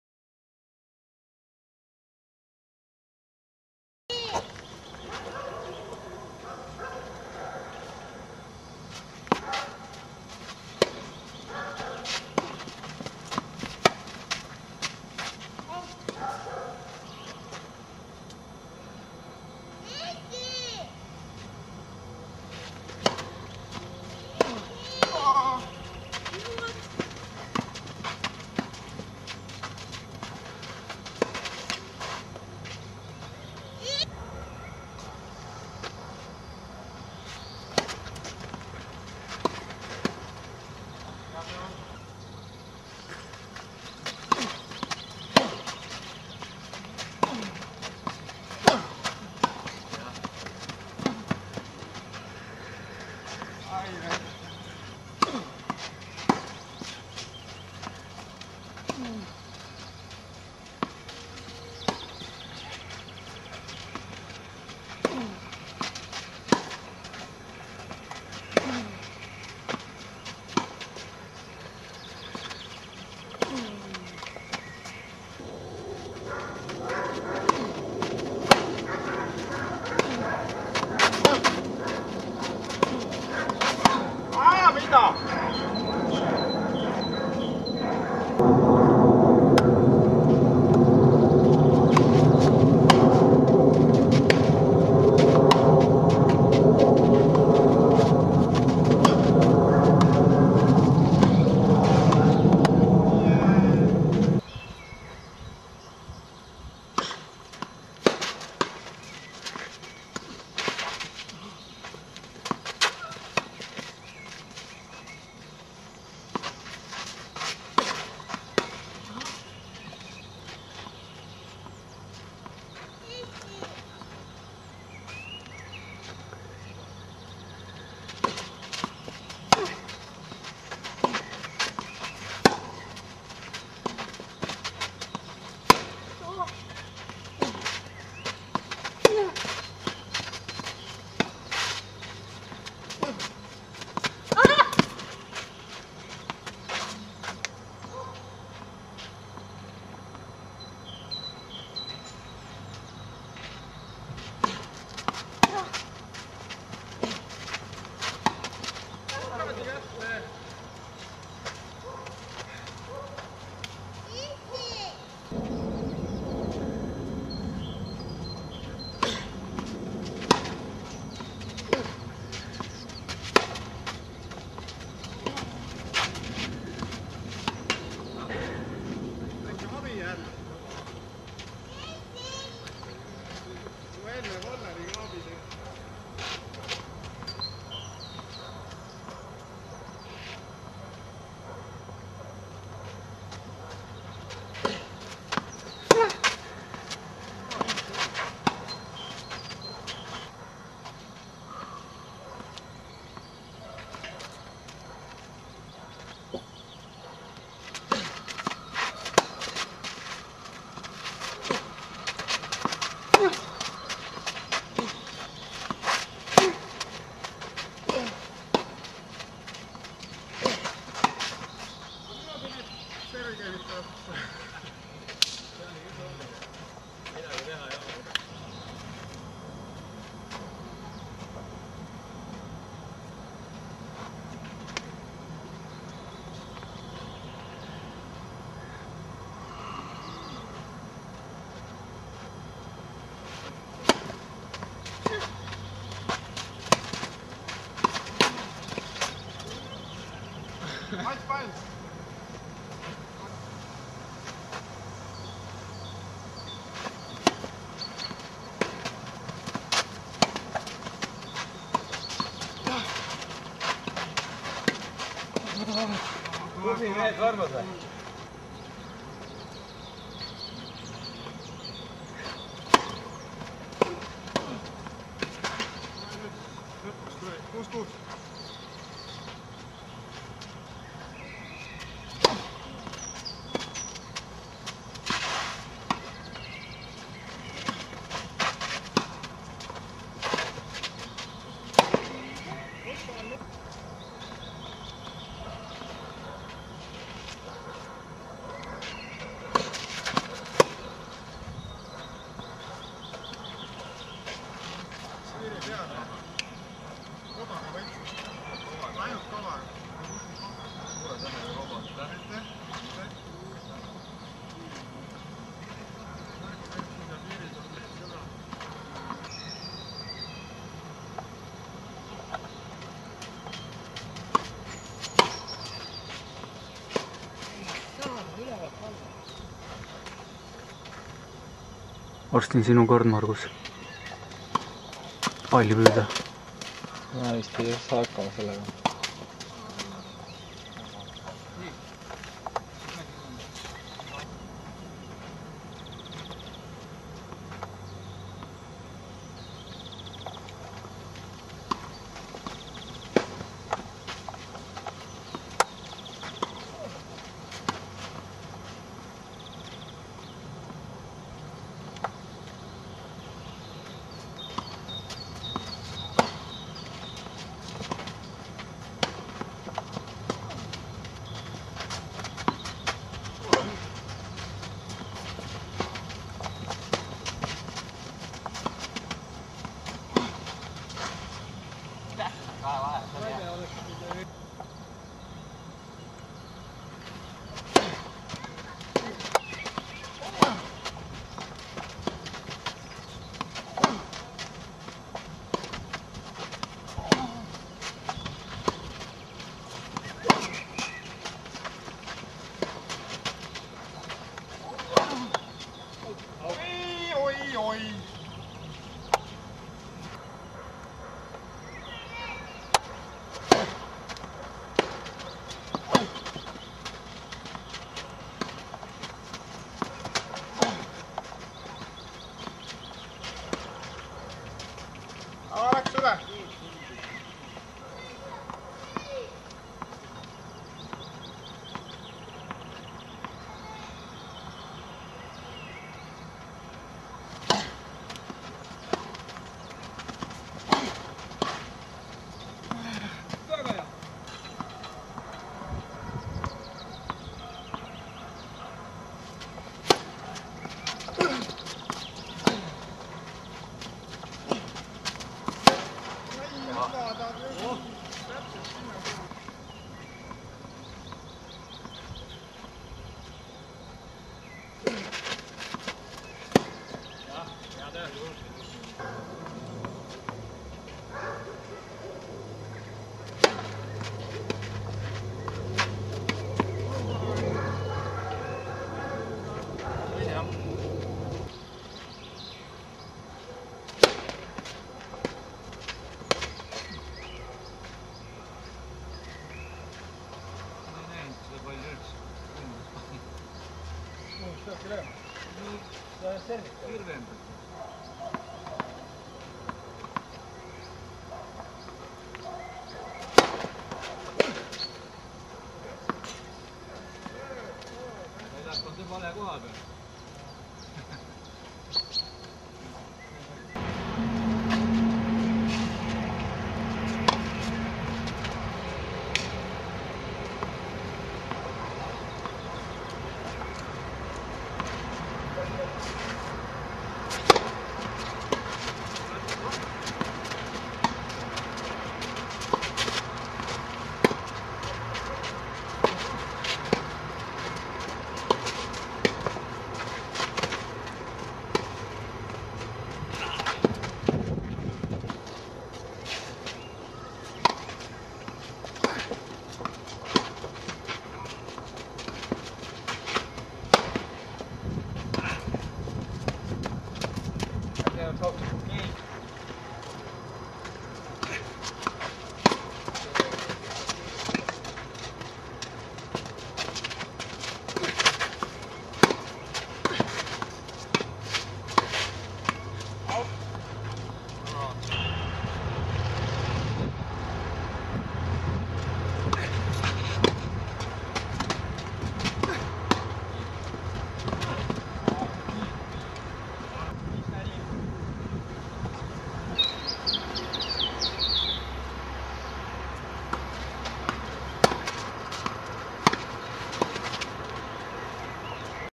TTU Kevad 2008 tennisevõistlused 1/2
TTU tennise hooaja avaturniir igamehe paarismängus, osa 1/2